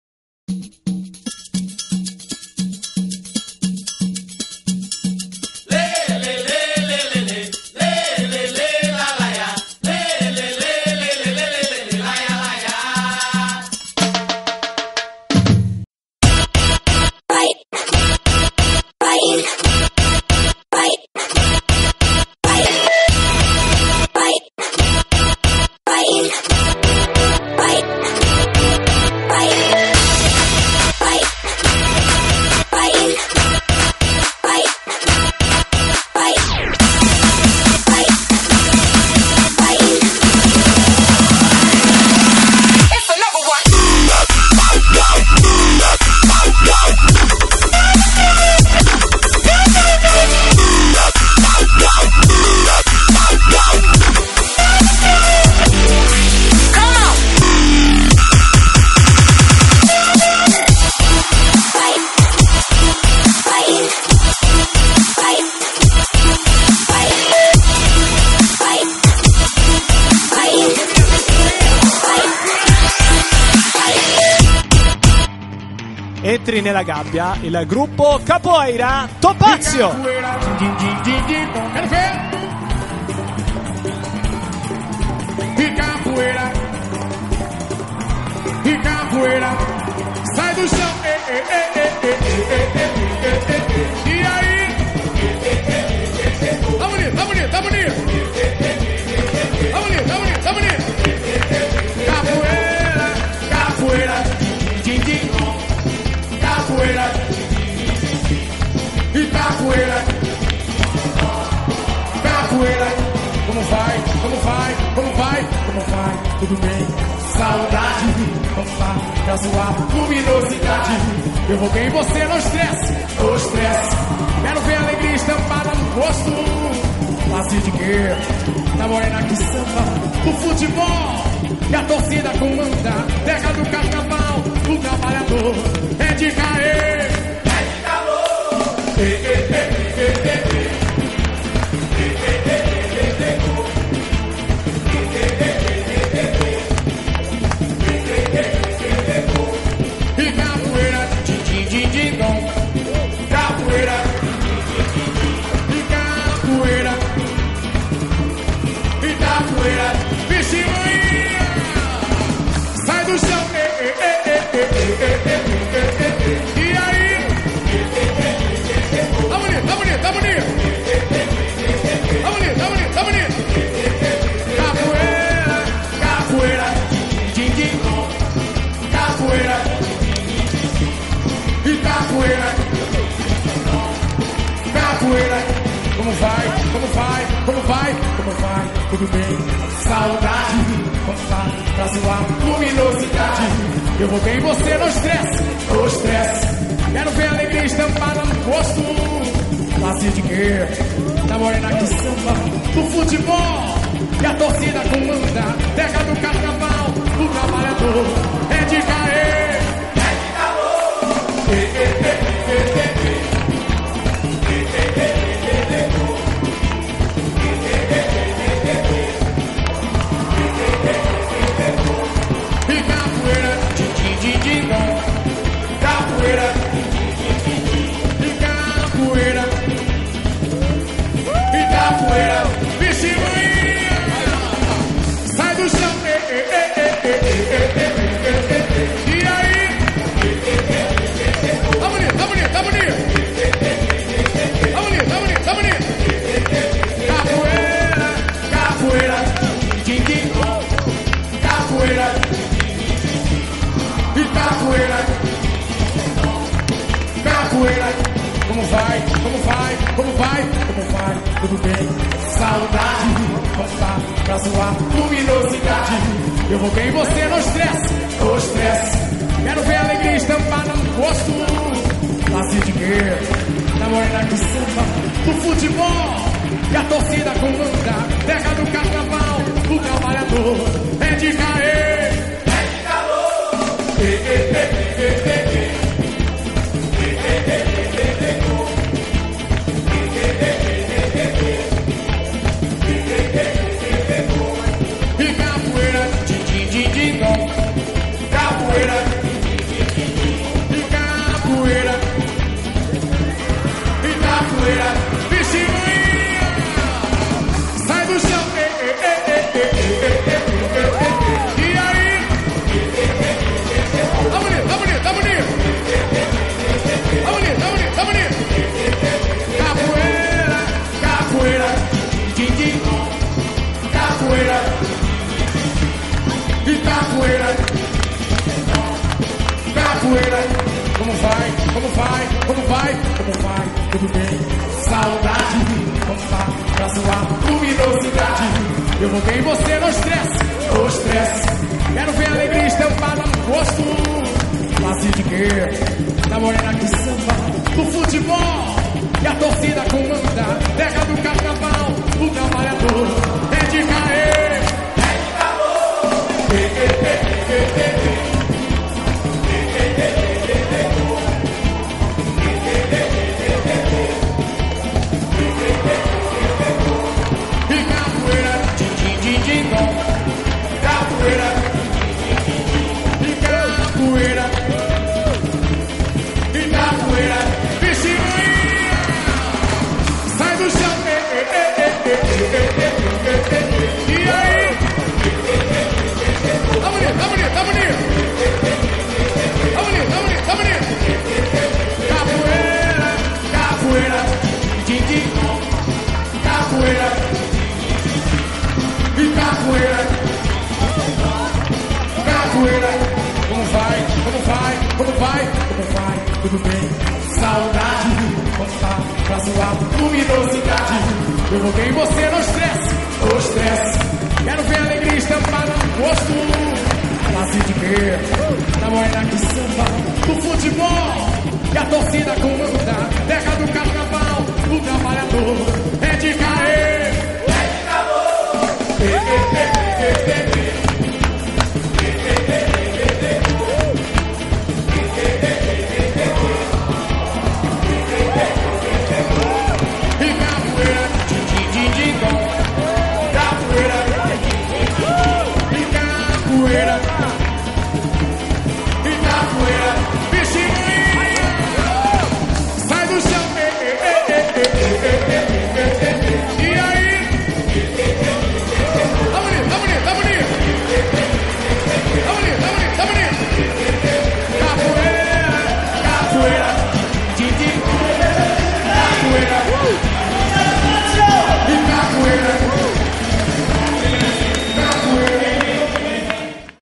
SAKARA IL LEGIONARIO e CAPOEIRA TOPAZIO
MMA Night event, Legio's Arena Roma 29/11/2013.
Evento promosso dal campione Alessio Sakara con la partecipazione speciale di Ricco Rodriguez.
Durante la serata di combattimenti nelle arti marziali miste il gruppo Capoeira Topazio si esibito in tecniche di Capoeira, Capo-Jitsu e Acrobatica.

Mestre Dinho
Mestre Agostinho
Prof. Neguinho
Prof. Tuquina
Instr. Lelé
Instr. Biel
Formada Roberica
Monitora Vania